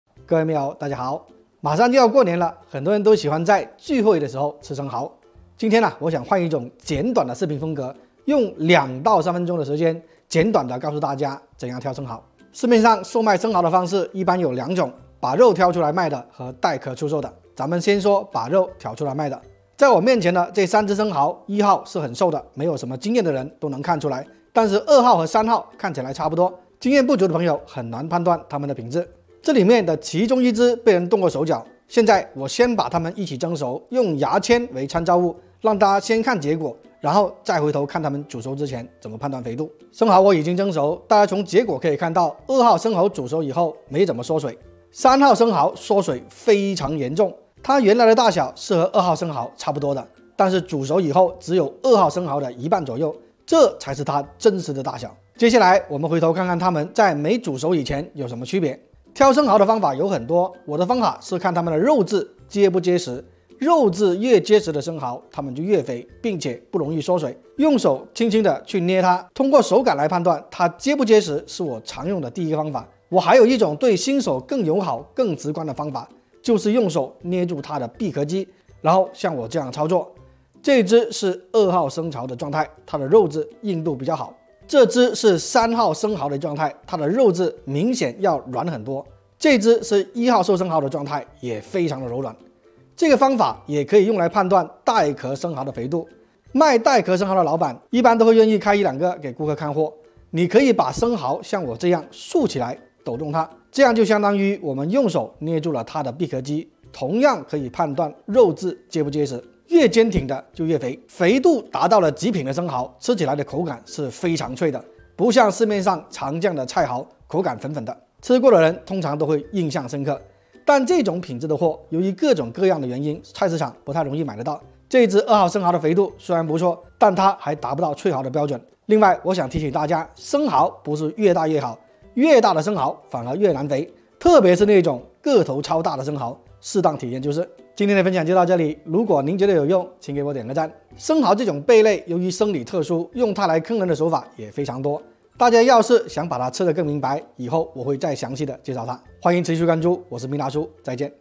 两分钟学会怎样挑选生蚝｜牡蛎｜海蛎
生蚝、牡蛎、海蛎怎样挑肥的。